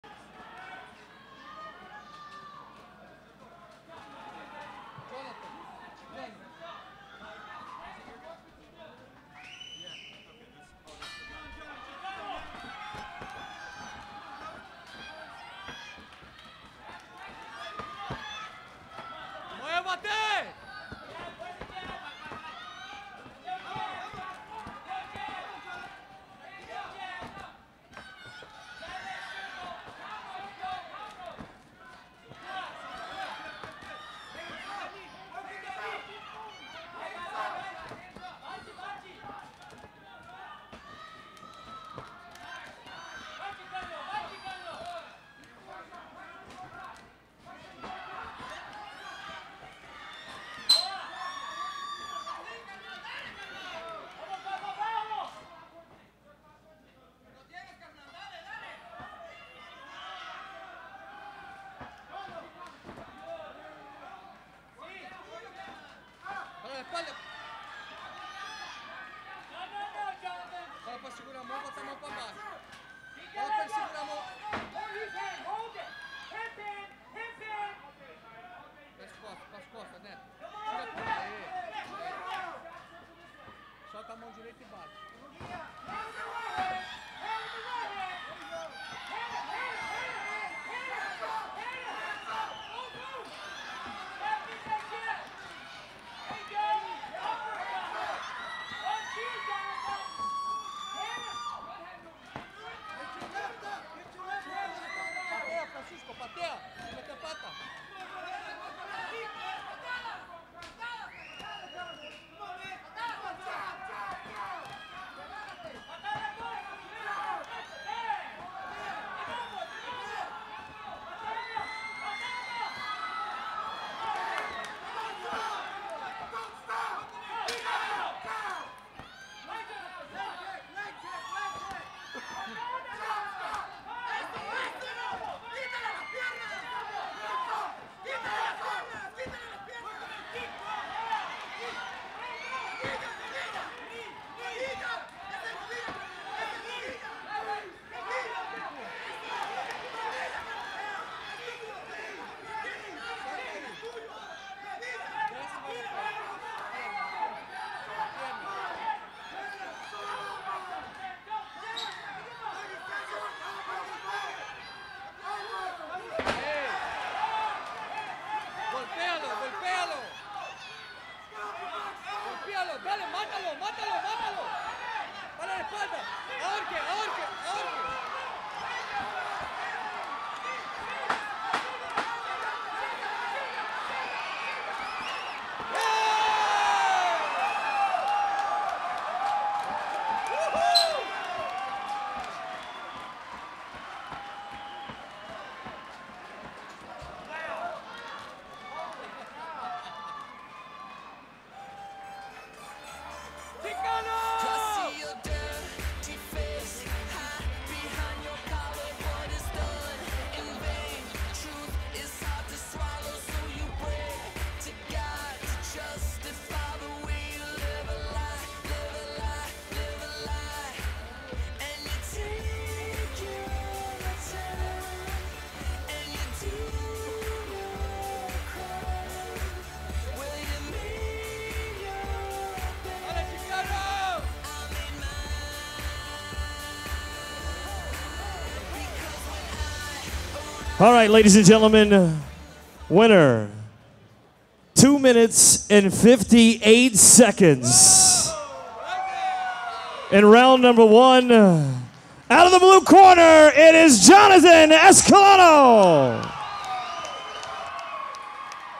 OO 13- Jonathan Escalona vs. Scott Oswald
OO 13 "Mixed Martial Arts Fighting Inside the Cage" Dulles Sportsplex, Sterling, VA, Saturday, October 30, 2010- Jonathan Escalona vs. Scott Oswald